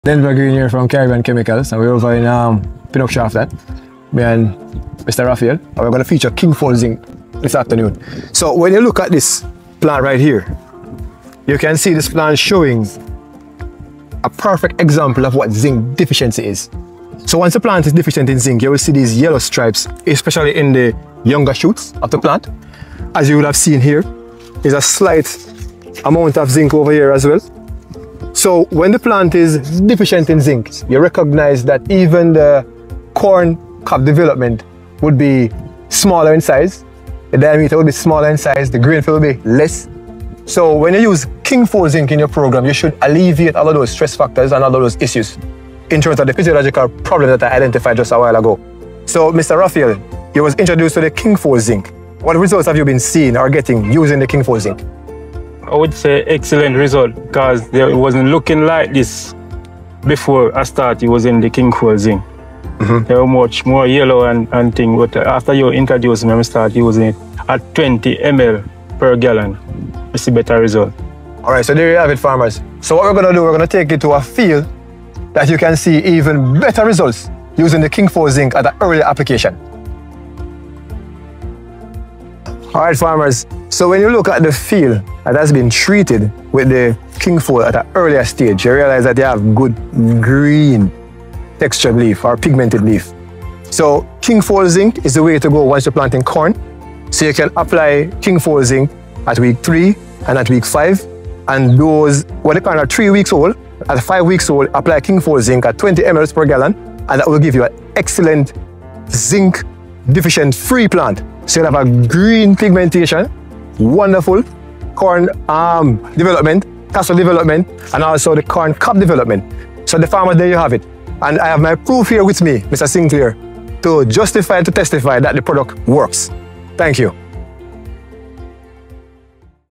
KingFol Zinc in Corn
Beating Zinc Deficiency in Corn!
Farmer Rafael Sinclair shares how Omex Kingfol Zinc, with guidance from Agronomist Denver Green, transformed his corn field, delivering healthier plants and stronger yields.

✅ Corrects zinc deficiency
✅ Boosts crop growth
✅ Supports higher productivity

Real results, straight from the field. 🌱